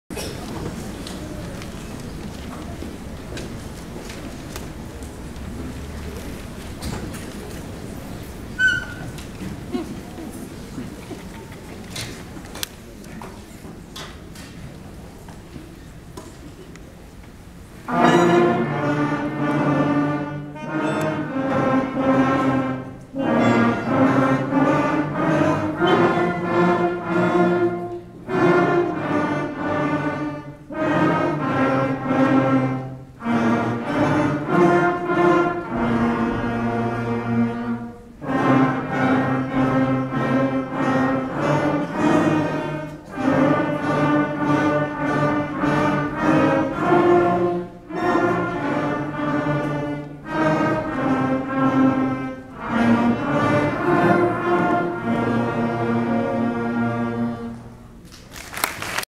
Clarke Middle School 6th Grade First Concert!! (4) The end..
10/6/2015 Wonderful experience for the sixth graders and their families :)